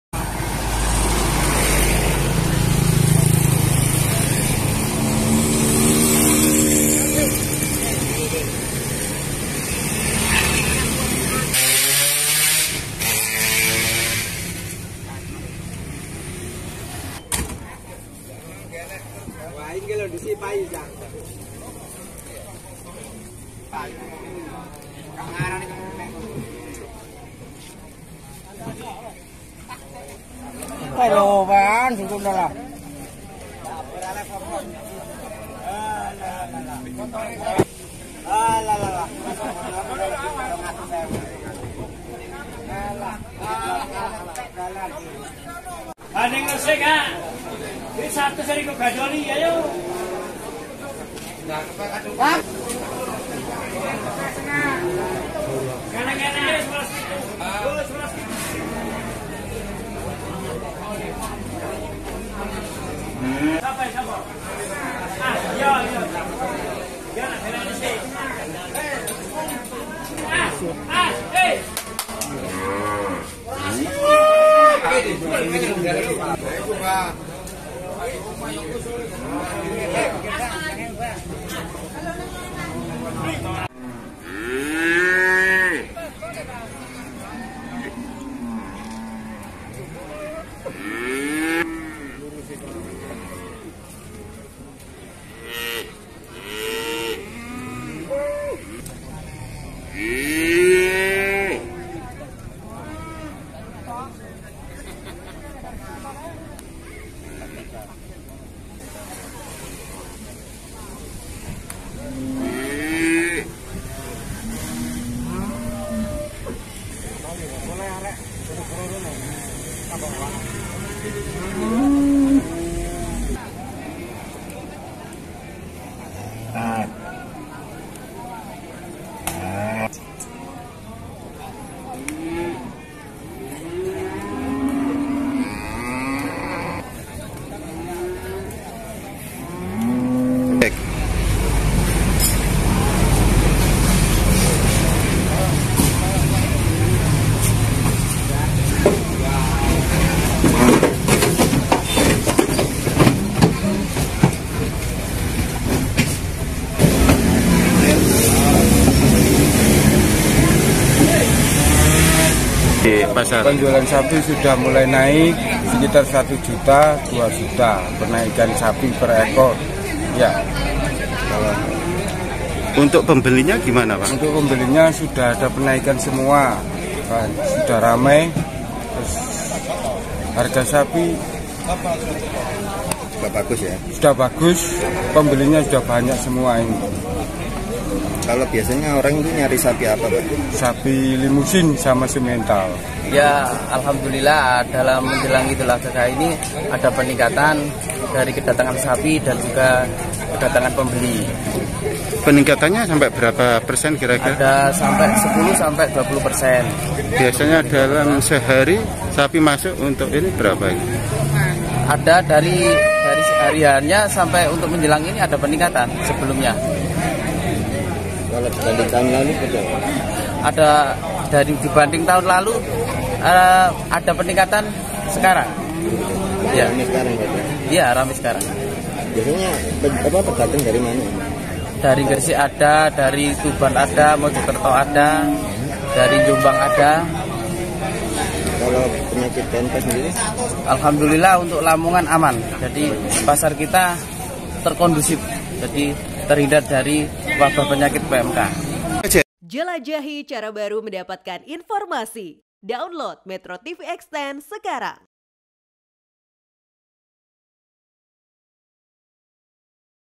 Naiknya Harga Sapi di Lamongan Tak Surutkan Minat Pembeli
METRO TV JATIM, Jelang hari raya kurban, harga sapi di Lamongan, Jawa Timur, naik Rp1-2 juta per ekor. Kendati demikian, kenaikan harga itu tak mengurangi minat para pembeli.